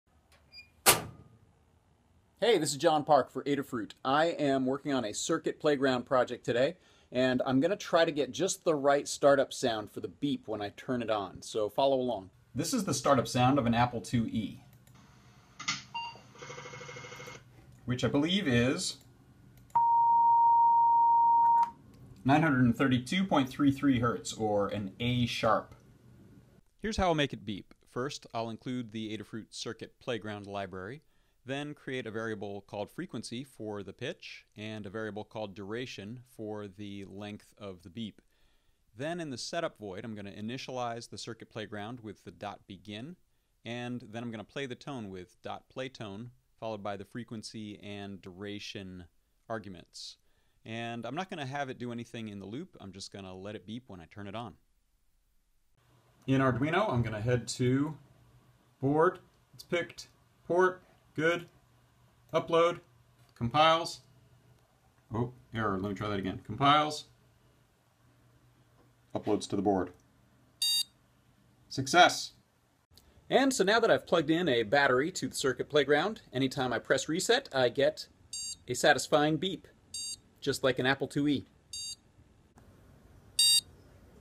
John Park's Circuit Playground with Apple IIe Startup Sound @adafruit @johnedgarpark
John Park creates a startup sound for the Circuit Playground dev board based upon the lovely Apple IIe beep.